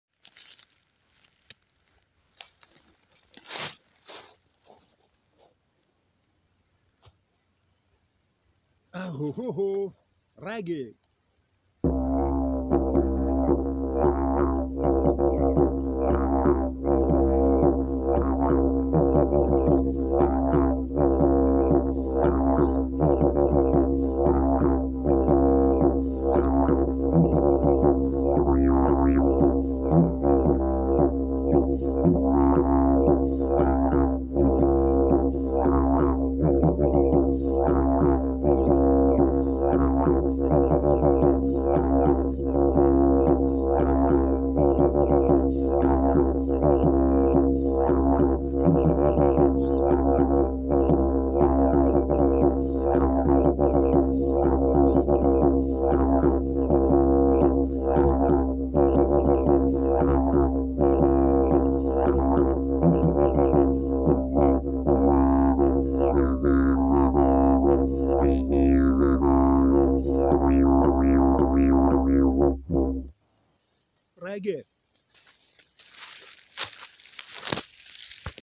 didjeridoo²² kind of reggae
Me in a private field where I could park my van ...now It has been a year since is without Number plate🙏